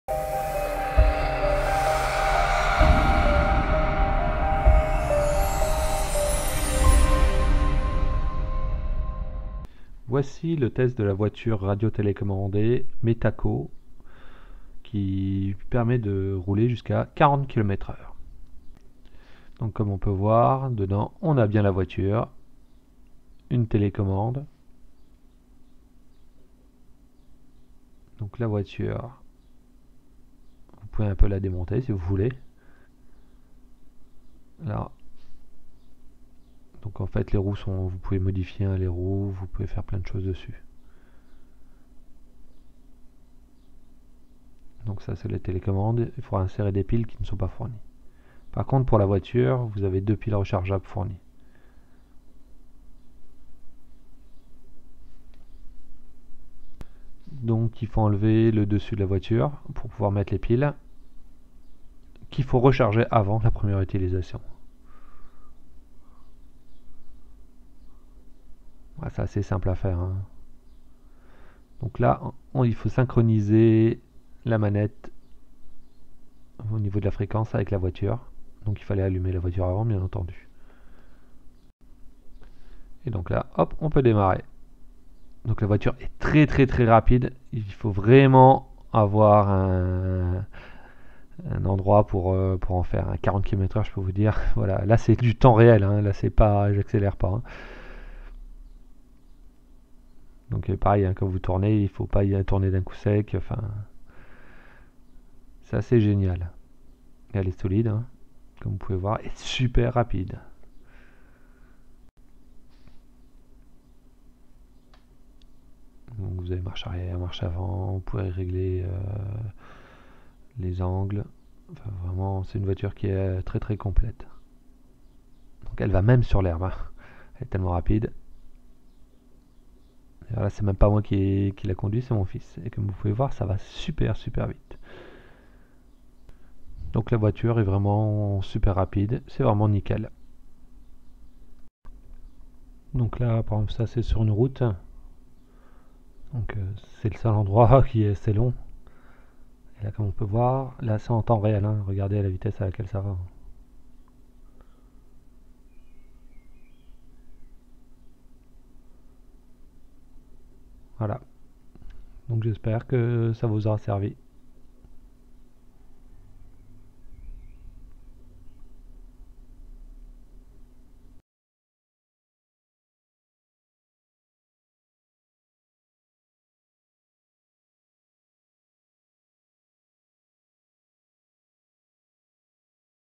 Test de la Buggy Hobby Car électrique 40 km/h de Metakoo
Test de la Buggy Hobby Car électrique qui roule à 40 km/h de Metakoo.
Cette voiture est destiné aux adultes ou adolescents. Pour les enfants plus jeune, c'est sous la surveillance d'un adulte :)